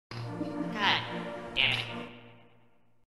Goddamnit [Re-animated SFM]
So, I remade Ben saying "God... dammit".